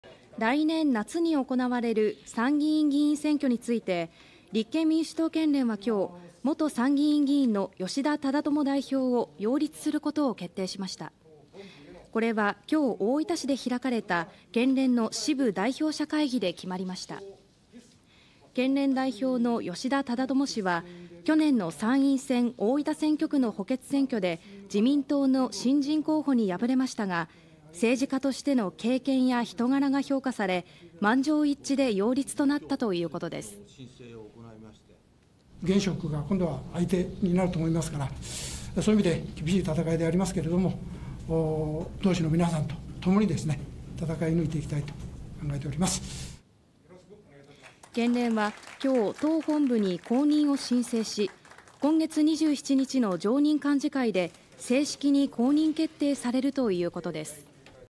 立憲民主党大分県連が吉田忠智代表の擁立決定 来年夏の参議院選挙 大分
来年夏に行われる参議院議員選挙について、立憲民主党大分県連は21日、元参議院議員の吉田忠智代表を擁立することを決定しました。

これは21日、大分市で開かれた県連の支部代表者会議で決まりました。
県連代表の吉田忠智氏は、去年の参院選大分選挙区の補欠選挙で自民党の新人候補に敗れましたが、政治家としての経験や人柄が評価され満場一致で擁立となったということです。

◆吉田忠智代表（67）
「現職が今度は相手になると思うから、そういう意味で厳しい戦いであるが、みなさんと共に戦い抜いていきたいと考えている」

県連は21日、党本部に公認を申請し、2月27日の常任幹事会で正式に公認決定されるということです。